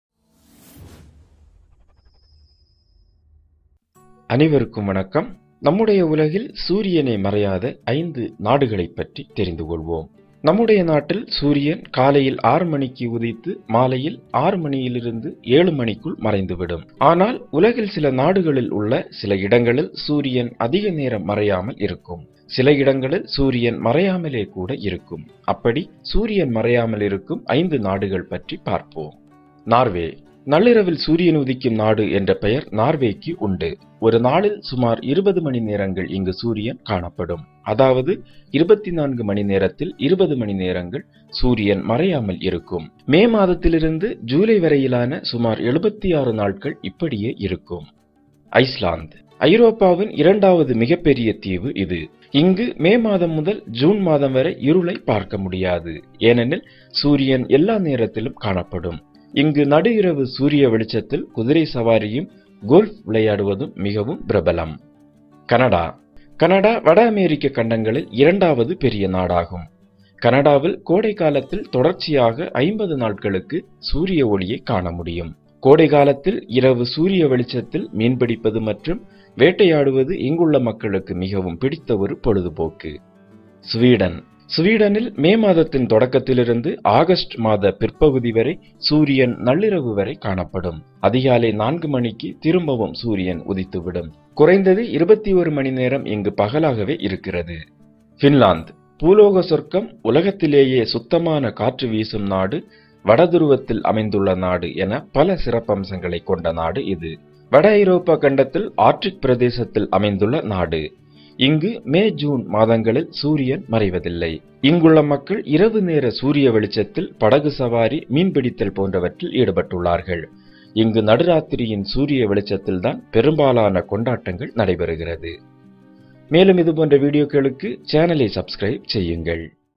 உலகில் சூரியனே மறையாத 5 நாடுகள்
நம்முடைய நாட்டில் சூரியன் காலையில் 6 மணிக்கு உதித்து மாலையில் 6 மணியிலிருந்து 7 மணிக்குள் மறைந்துவிடும். ஆனால் உலகில் சில நாடுகளில் உள்ள சில இடங்களில் சூரியன் அதிக நேரம் மறையாமல் இருக்கும். சில இடங்களில் சூரியன் மறையாமலே கூட இருக்கும். அப்படி சூரியன் மறையாமல் இருக்கும் 5 நாடுகளின் இடங்களை பற்றி தெரிந்துகொள்வோம் வாருங்கள்